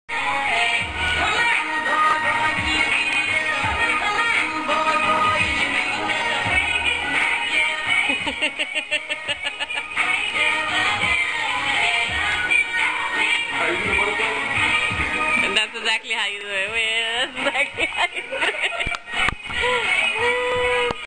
So you think you can dance
Uncle Raul and Lana blow up the dance floor